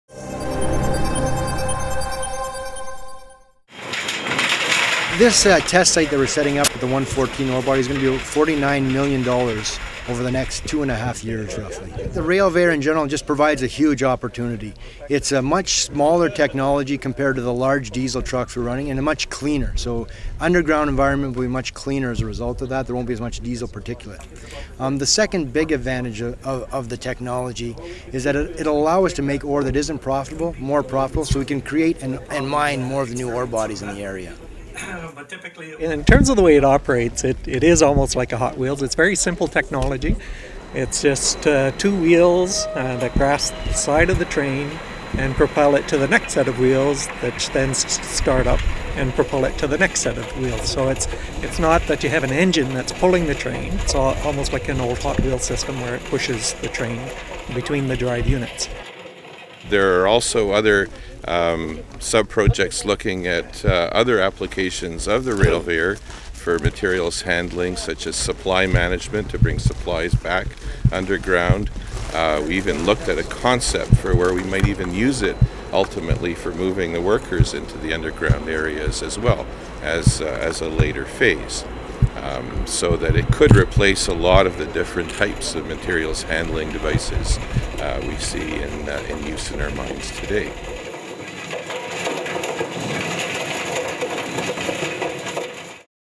Rail-veyor offers efficient mining technology - Sudbury News
Sudbury - The rail-veyor is designed to convey materials and supplies underground. This new technology is being tested. This technology will offer opportunities to test safer and more efficient mining techniques.